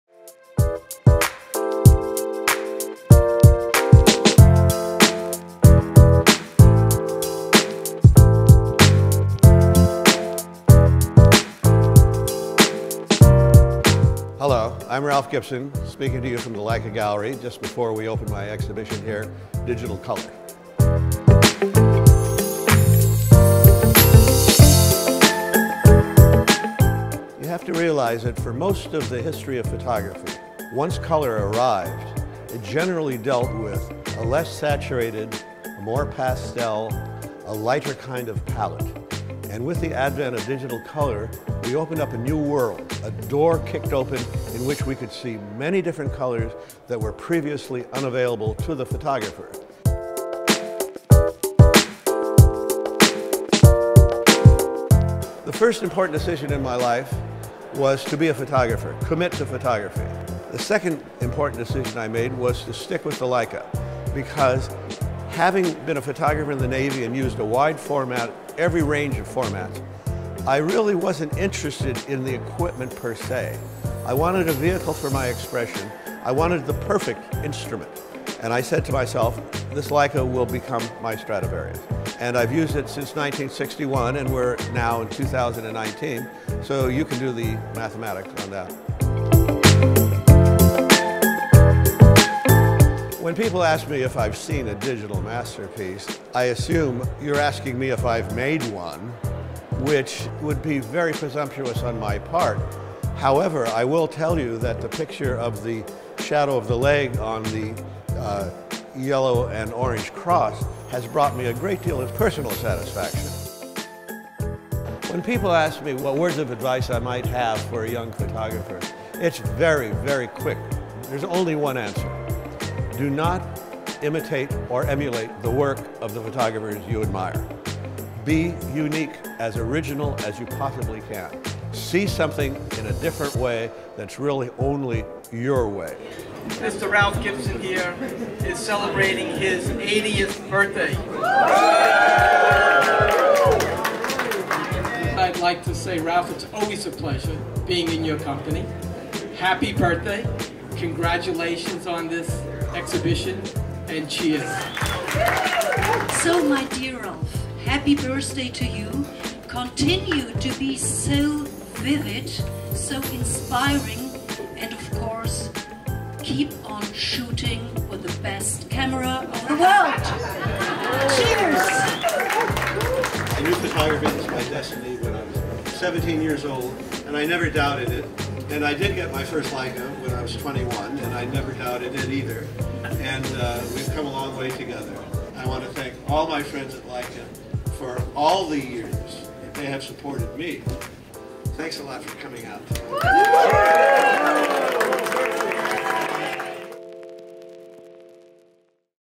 Ralph Gibson's "Digital Color" Exhibition & 80th Celebration
On January 17th, 2019, Leica celebrated the 80th milestone birthday of Leica legend, Ralph Gibson, coinciding with his opening reception of "Digital Color" at Leica Gallery Los Angeles.

The exhibition featured a series of captivating digital photographs taken by Gibson, who solely used a Leica Rangefinder and a 135mm Apo-Telyt-M lens.